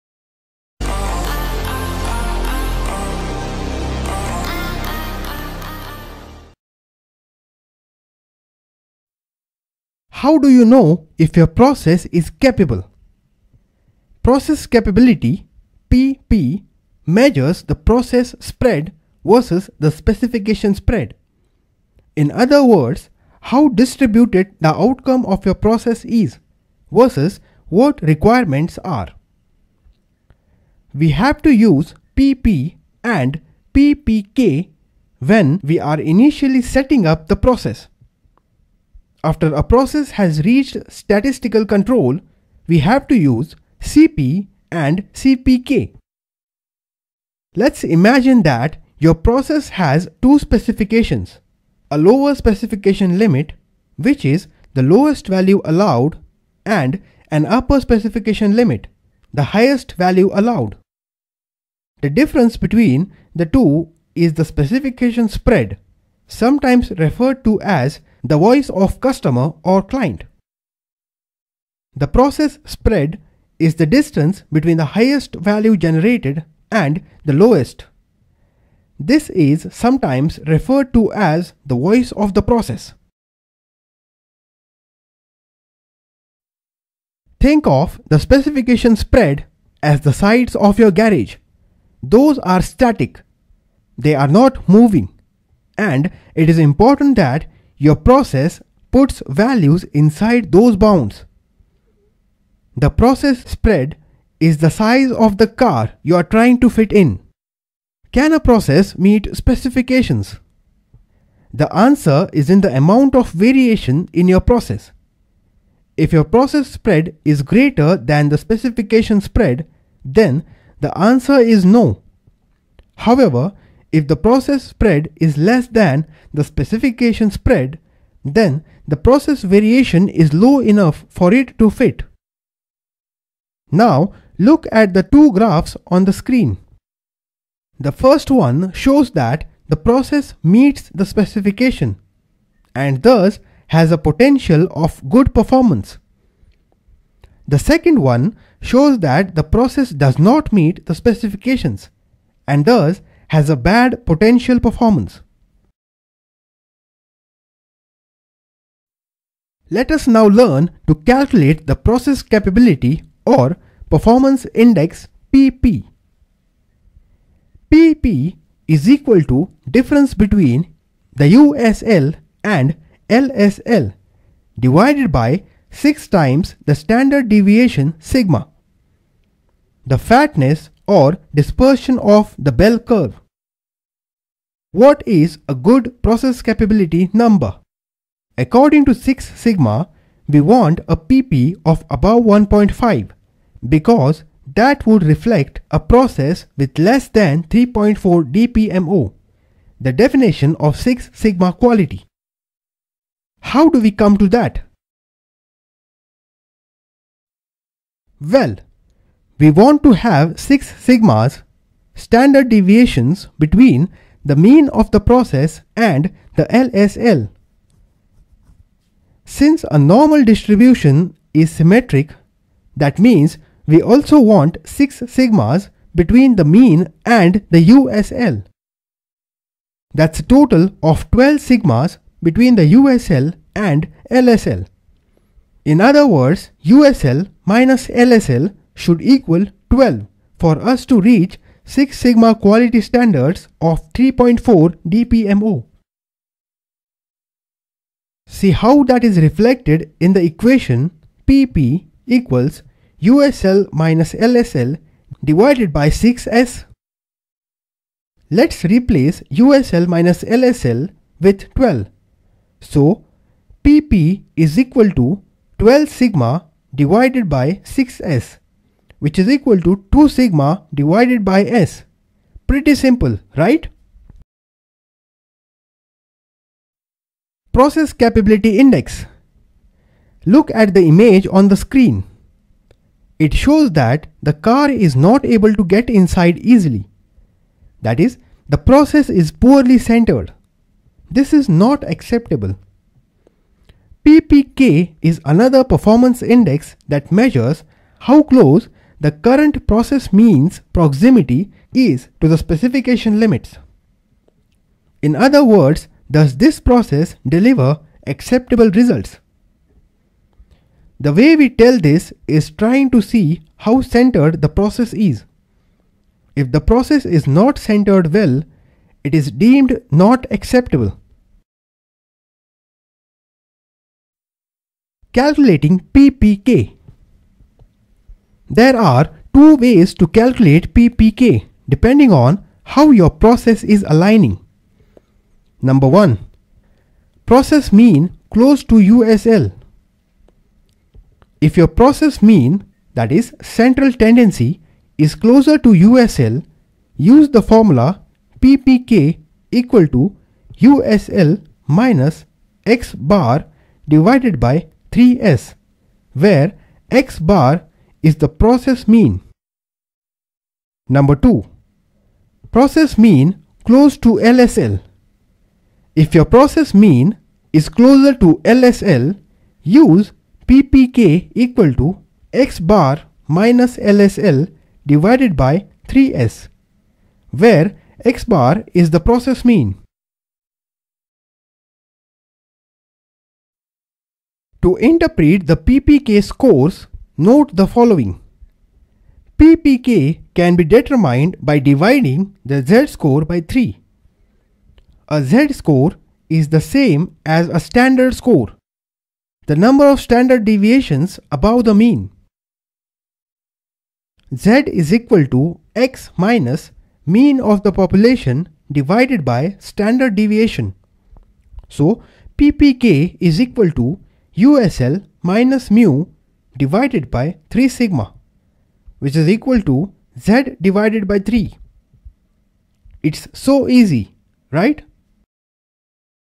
Six Sigma Training Videos | Process Performance Pp & Ppk | ACTSol & Associates
This is a small video on Process Performance Pp & Ppk.

It is a part of the eLearning course on Six Sigma, offered by ACTSol & Associates

Intro Music Credits - OurMusicBox - Jayman